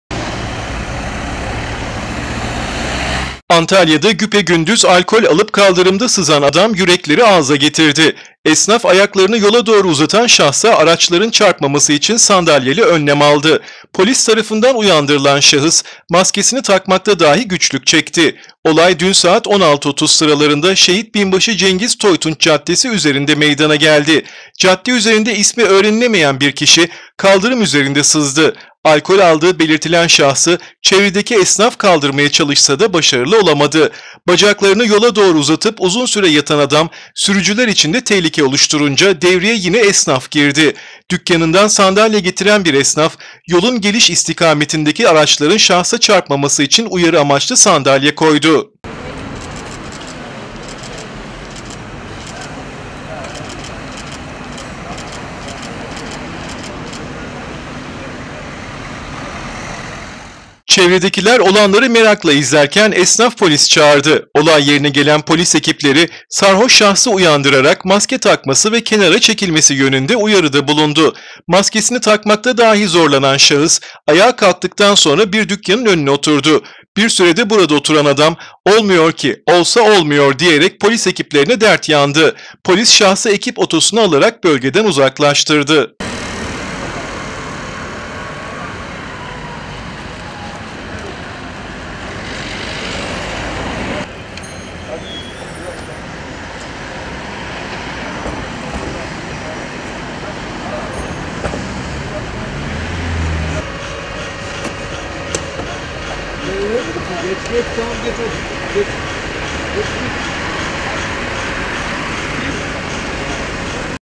SARHOŞ ADAMI ESNAF KORUDU
Antalya’da güpegündüz alkol alıp kaldırımda sızan adam yürekleri ağza getirdi. Esnaf, ayaklarını yola doğru uzatan şahsa araçların çarpmaması için sandalyeli önlem aldı.